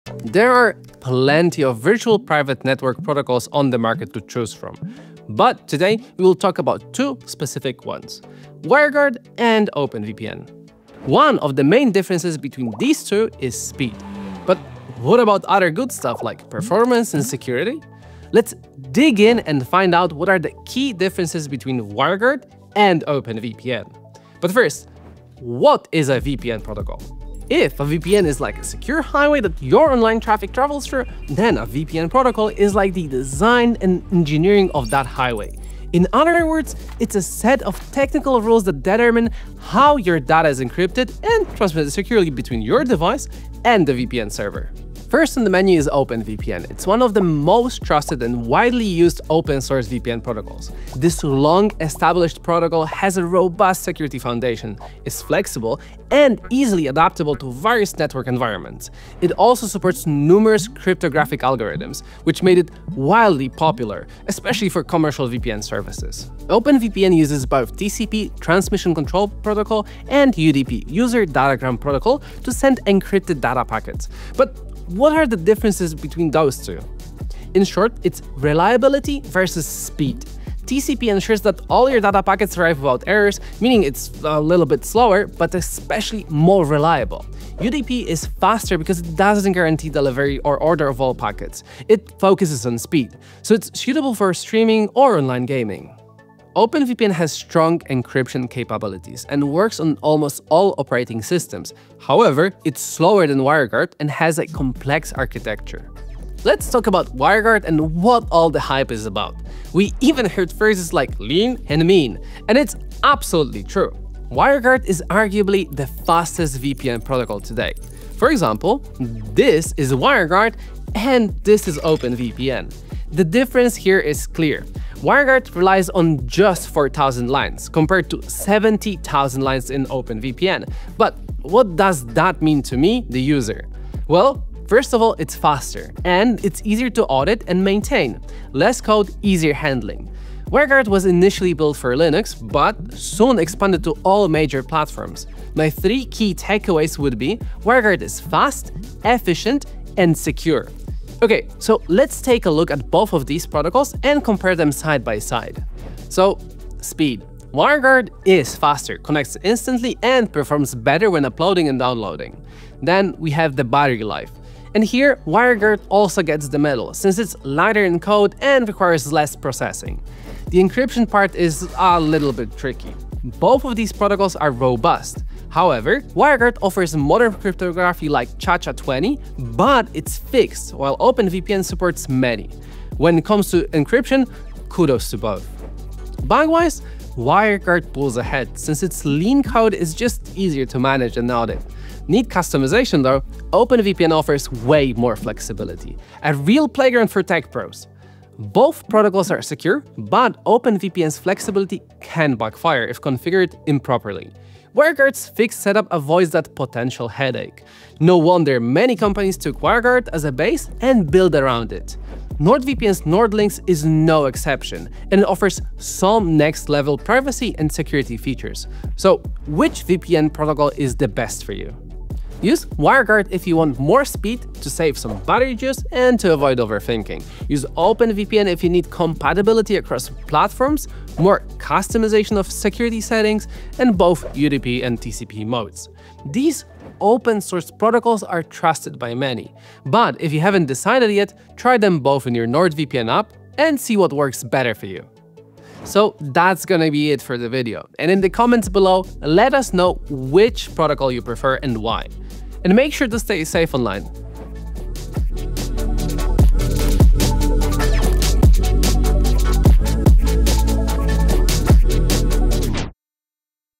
WireGuard or OpenVPN: Which protocol should you use in 2025?
WireGuard or OpenVPN protocol? 🤓
Easy online protection at home and on the go 🧘​

There are plenty of virtual private network protocols on the market. But today, we will discuss two: WireGuard and OpenVPN. Find out the key differences between them and which one works best for you.

► RECOMMENDED VIDEOS ►

Contents:

00:00 Intro
00:26 What is a VPN protocol
00:47 OpenVPN protocol explained
01:46 WireGuard protocol explained
02:32 OpenVPN vs WireGuard
03:39 The best VPN protocol for you

There are plenty of virtual private network protocols on the market to choose from. But today, we will talk about two: WireGuard and OpenVPN. One of the main differences between these two is speed. But what about other good stuff, like performance and security? Let’s dig in and find out what are the key differences between WireGuard and OpenVPN.

But what is a VPN protocol?

If a VPN is like a secure highway that your online traffic travels through, then a VPN protocol is like the design and engineering of that highway. In other words, it’s a set of technical rules that determine how your data is encrypted and transmitted securely between your device and the VPN server.

First on the menu is OpenVPN. It’s one of the most trusted and widely used open-source VPN protocols. This long-established protocol has a robust security foundation, is flexible, and easily adaptable to various network environments. It also supports numerous cryptographic algorithms, which made it wildly popular, especially for commercial VPN services.

OpenVPN uses both TCP — Transmission Control Protocol,, and UDP — User Datagram Protocol to send encrypted data packets. But what are the differences between those? In short, it’s reliability versus speed. TCP ensures that all your data packets arrive without errors, meaning it’s a bit slower but reliable. UDP is faster because it doesn’t guarantee delivery or order of all packets; it focuses on speed. So it’s suitable for streaming or online gaming. OpenVPN has strong encryption capabilities and works on almost all operating systems. However, it’s slower than WireGuard and has a complex architecture.

Let’s talk about WireGuard and what all the hype is about. We even heard phrases like “lean and mean.” And it’s absolutely true! WireGuard is arguably the fastest VPN protocol today. For example, this is Wireguard and OpenVPN. The difference is clear. WireGuard relies on just 4,000 lines, compared to 70k lines in OpenVPN. But what does that mean to me, the user? Well, it’s faster, easier to audit and maintain. Less code — easier handling! WireGuard was initially built for Linux but soon expanded to all major platforms. My three key takeaways would be: Wireguard is fast, efficient, and secure.

Okay, let’s take a look at both of these protocols and compare them side-by-side.

Speed. Wireguard is faster, connects instantly, and performs better when uploading and downloading.
Then we have the battery life. WireGuard here also gets the medal since it’s lighter in code and requires less processing.
The encryption part is tricky. Both of these protocols are robust. WireGuard offers modern cryptography like Chacha20, but it’s fixed, while OpenVPN supports many. When it comes to encryption, kudos to both!

“Bugwise,” WireGuard pulls ahead, since its lean code is just easier to manage and audit. Need customization, though? OpenVPN offers way more flexibility, a real playground for tech pros! Both protocols are secure, but OpenVPN's flexibility can backfire if configured improperly. WireGuard's fixed setup avoids that potential headache. No wonder many companies took WireGuard as a base and built around it. NordVPN’s NordLynx is no exception, and it offers some next-level privacy and security features.

So which VPN protocol is best for you?

Use WireGuard if you want more speed, to save some battery juice, and to avoid overthinking.
Use OpenVPN if you need compatibility across platforms, more customization of security settings, and both UDP and TCP modes.

These open-source protocols are trusted by many. But if you haven’t decided yet, try them both in your NordVPN app and see what works better for you. In the comments, let us know which one you prefer and why. Stay safe online.